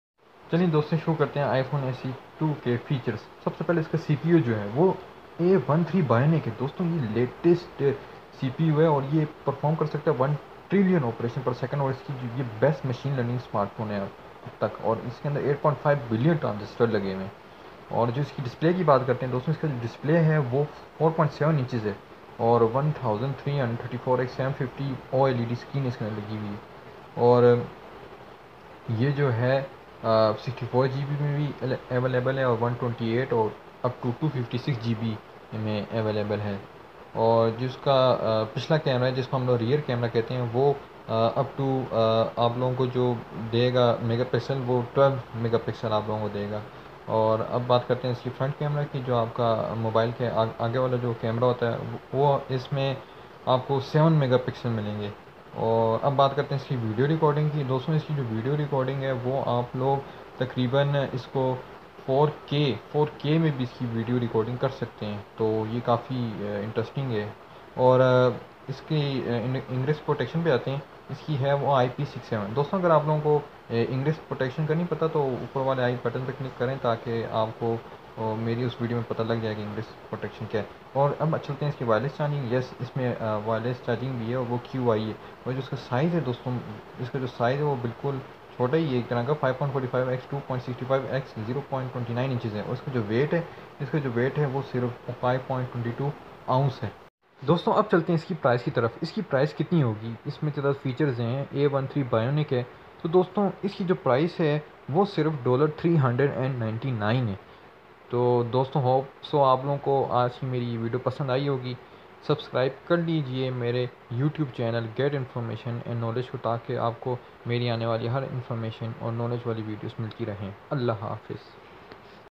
iPhone se2 ka kia features hain
Dekhain is video main iPhone se2 ka features kia kia hain.

iPhone se2 features by GET INFORMATION AND KNOWLEDGE iPhone Se2 iPhoneSe2 features #2020